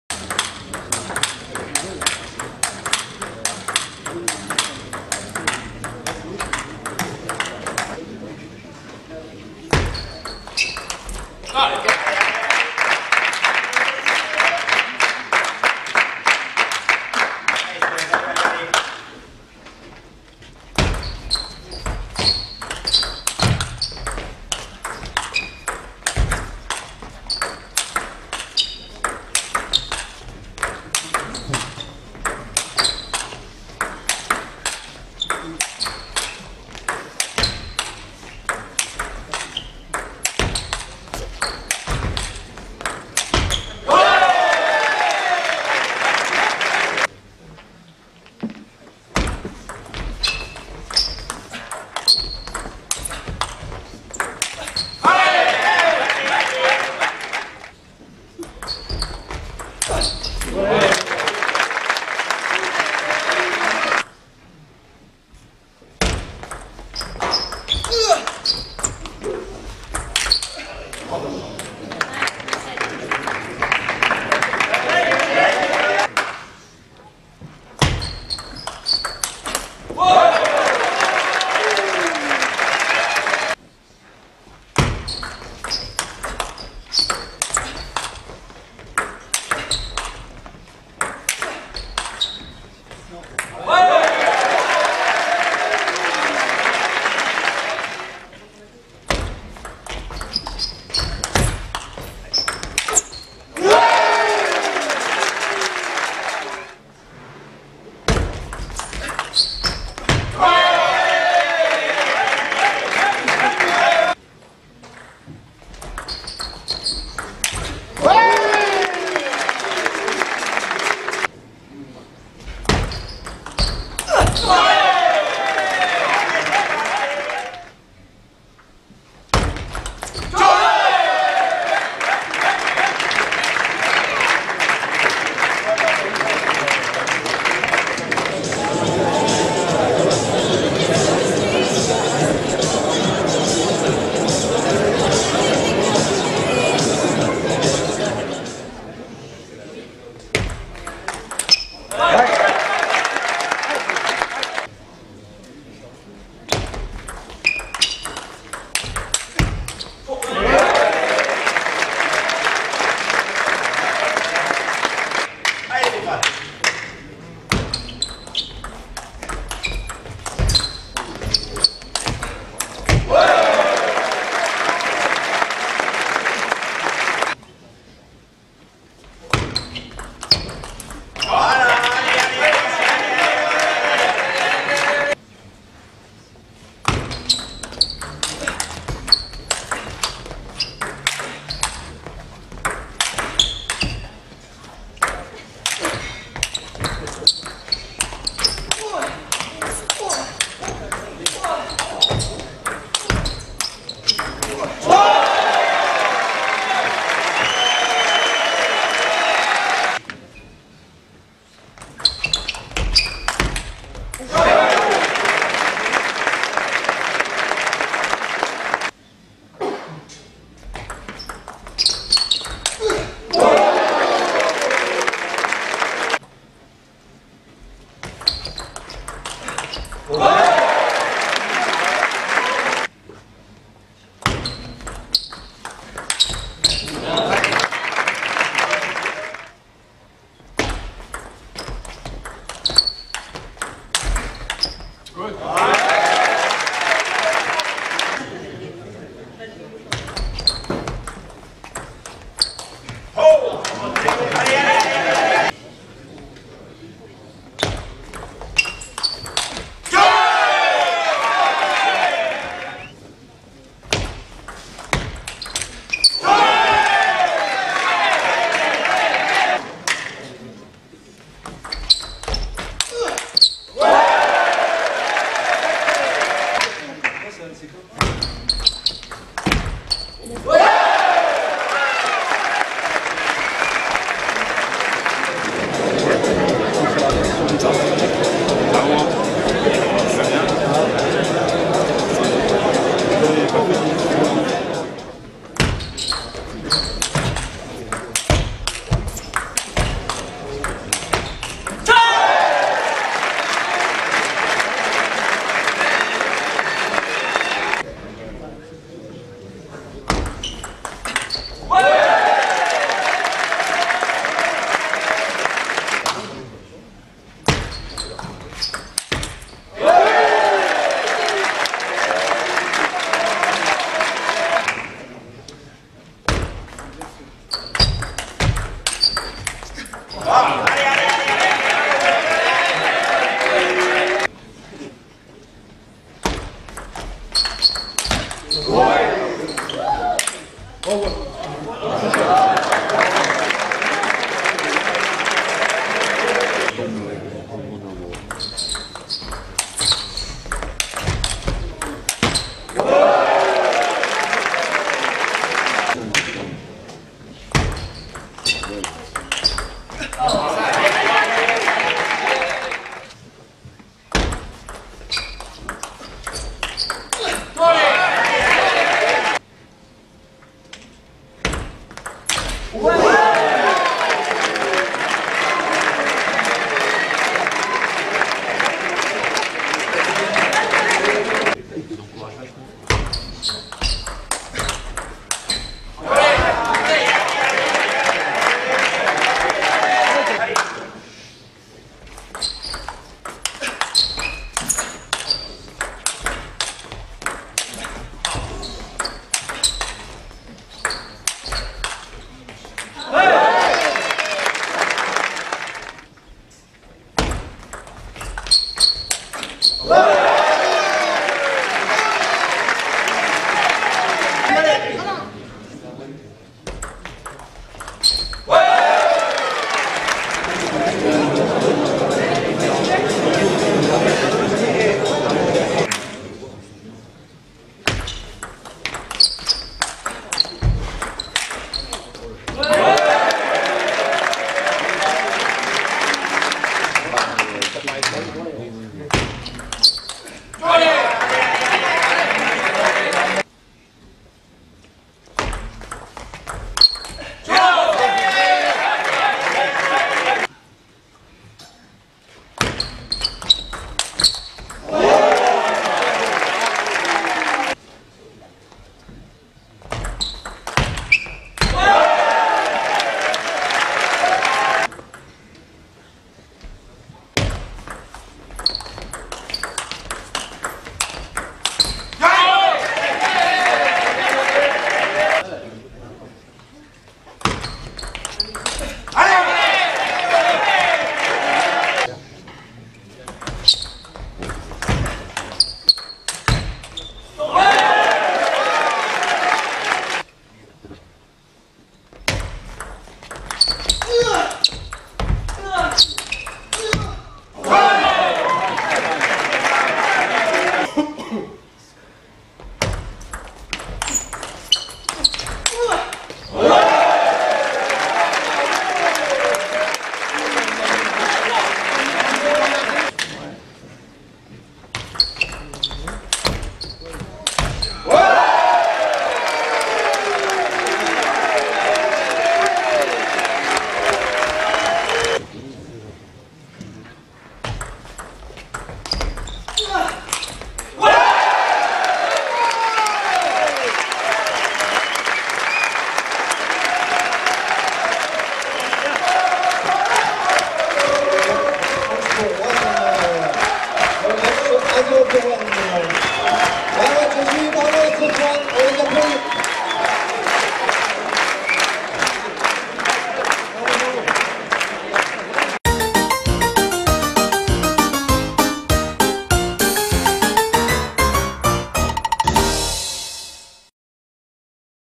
WU ZHANG Jiaji - Stéphane OUAICHE (momo rétro)
Extraits match Pro A  2017  LOIRE NORD TT / PPC VILLENEUVE à Roanne